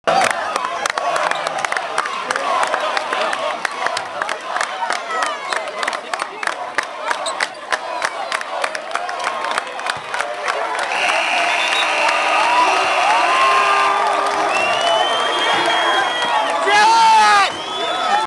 Cohoes Stomps 'Burgh
The closing seconds of the Cohoes Tigers' 52-37 stomping of Lansingburgh.  1/16/07